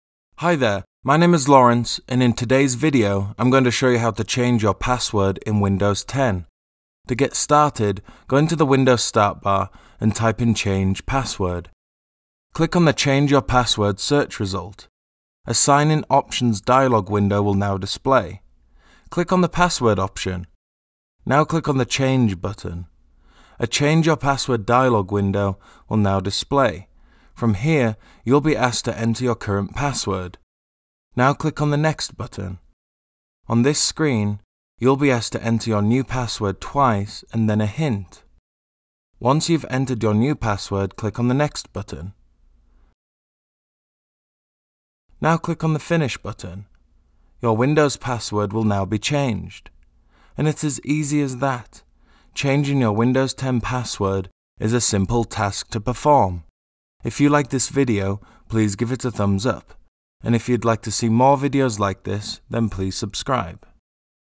How to Change Password in Windows 10
In today’s video, I’m going to show you how to change your password in Windows 10.

1. Go into the Windows start bar, and type in “Change Password”.
2. Click on the “Change your password” search result.
3. A “Sign-in options” dialog window will now display.
4. Click on the “Password” option.
5. Now click on the “Change” button.
6. A “Change your password” dialog window will now display. From here you’ll be asked to enter your current password.
7. Now click on the “Next” button.
8. On this screen, you’ll be asked to enter your new password twice, and then a hint.
9. Once you’ve entered your new password, click on the “Next” button.
10. Now click on the “Finish” button.
11. Your Windows password will now be changed.

And it’s as easy as that, changing your Windows 10 password is a simple task to perform.

*Regularly changing your password in Windows is a good security practice for several reasons:*

*Mitigating Security Risks:*
- Regular password changes help mitigate the risk of unauthorized access. - If your password is compromised or if someone gains access to it, changing it regularly limits the window of opportunity for potential attackers.

*Protecting Sensitive Information:*
- Windows passwords are the primary defense against unauthorized access to your personal or business data. Changing your password regularly adds an extra layer of protection to sensitive information stored on your computer.

*Preventing Unauthorized Account Access:*
- If you use the same password across multiple accounts, changing it regularly reduces the chances of unauthorized access to other accounts in case one set of credentials is compromised.

*Meeting Security Compliance Standards:*
- In certain industries and organizations, regular password changes are a requirement to comply with security standards and regulations. Adhering to these standards helps maintain a secure computing environment.

*Protecting Against Credential Stuffing:*
- Credential stuffing is a cyber attack method where attackers use username and password combinations leaked from other data breaches to gain unauthorized access. Regularly changing your password helps protect against such attacks.

*Employee Turnover and Access Control:*
- In a workplace environment, employees may leave the organization or change roles. Regular password changes ensure that former employees or individuals with changed responsibilities no longer have access to sensitive company information.

*Reducing the Impact of Phishing:*
- Phishing attacks often involve tricking users into revealing their passwords. Even if you fall victim to a phishing attempt, changing your password regularly limits the time attackers can exploit the compromised credentials.

*Enhancing Overall Security Hygiene:*
- Regularly changing your password is a good cybersecurity habit. It encourages users to stay vigilant about their security practices, promoting overall security hygiene and awareness.

*Password Aging Policies:*
- Some organizations implement password aging policies that require users to change their passwords after a specified period. This is done to ensure that passwords remain dynamic and less susceptible to prolonged attacks.

*Password Complexity and Strength:*
- When changing your password, it's an opportunity to create a strong and complex password. Regular changes encourage users to adopt robust password practices, such as using a combination of uppercase and lowercase letters, numbers, and special characters.
- While changing passwords is beneficial, it's equally important to create strong, unique passwords for each account, enable multi-factor authentication where possible, and stay vigilant against phishing attempts to maintain a robust security posture.